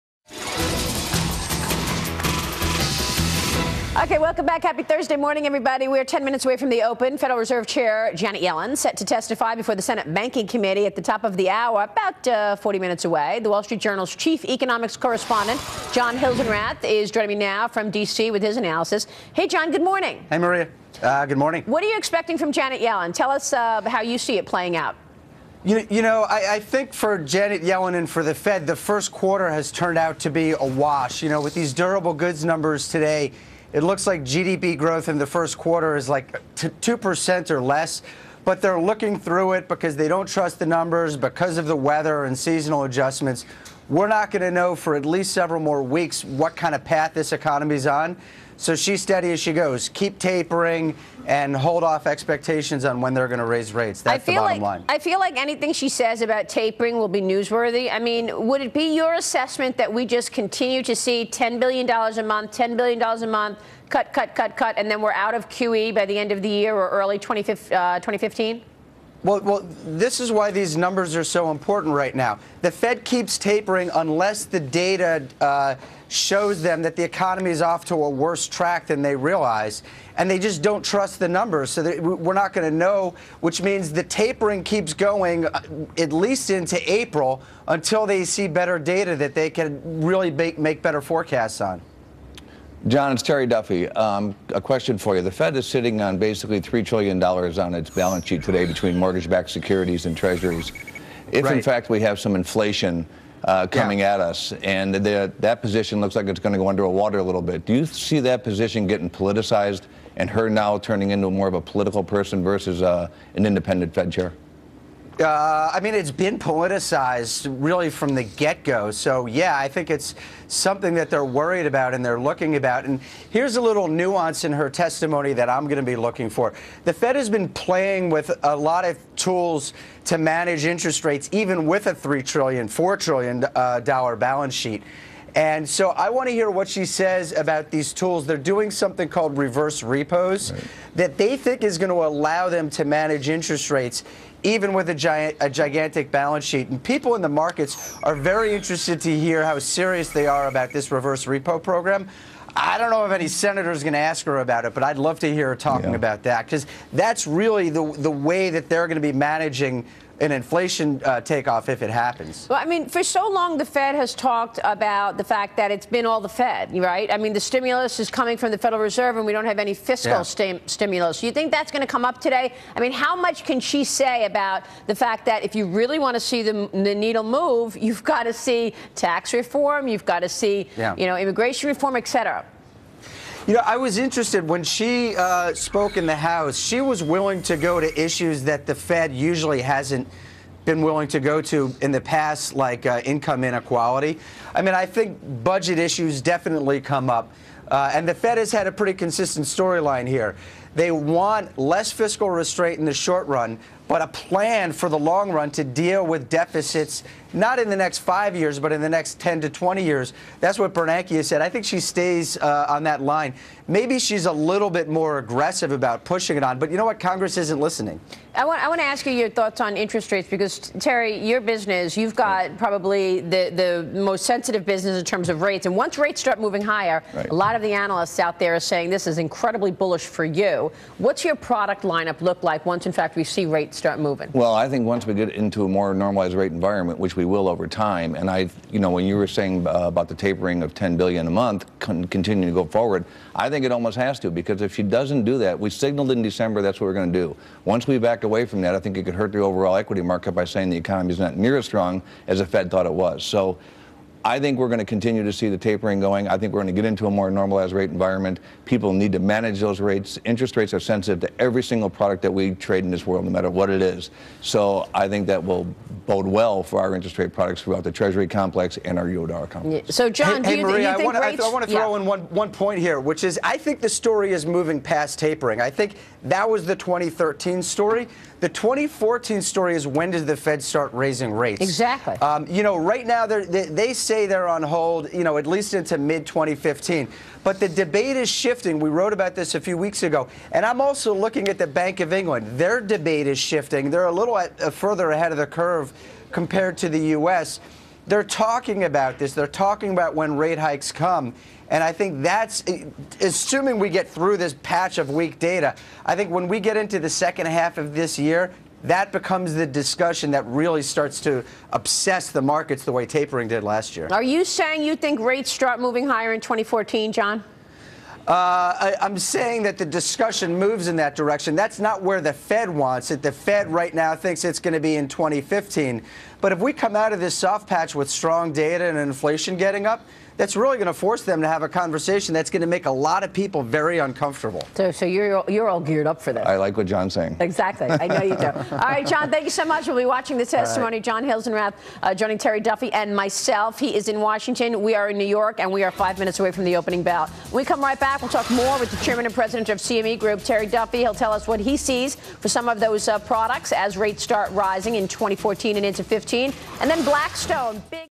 Terry Duffy Discusses Janet Yellen and the Federal Reserve
Terry Duffy, Executive Chairman and President, CME Group